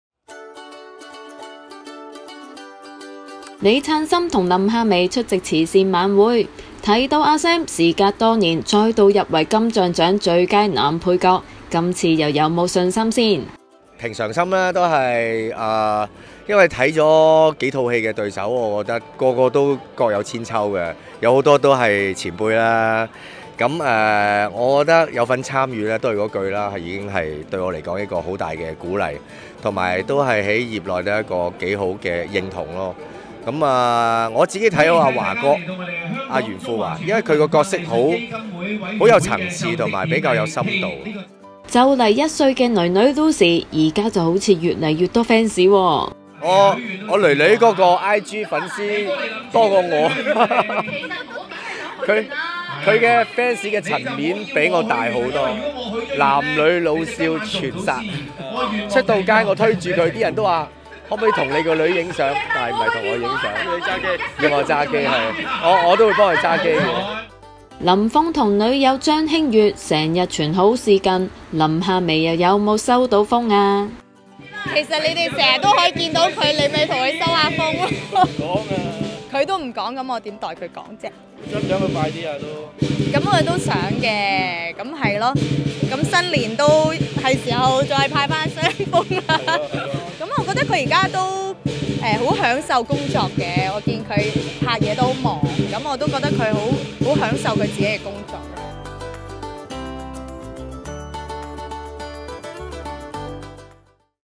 【娛樂】李璨琛囡囡人氣勁 林夏薇盼林峯早日結婚 2019-03-04
李璨琛和林夏薇出席慈善晚會，提到阿Sam事隔多年，再度入圍金像獎最佳男配角，他說今年的對手各有千秋，將會以平常心面對。

提到囡囡的人氣勁，他更自爆他們上街的時候，不少人都是找他的囡囡拍照，而不是，甚至要幫人拿相機，笑言囡囡的fans層面甚廣。林峯與女友張馨月成日傳好事近，不知道林夏薇有沒有收到風呢？她說︰「我都想，都係時候新年派雙封啦！」

bmgproductionmusic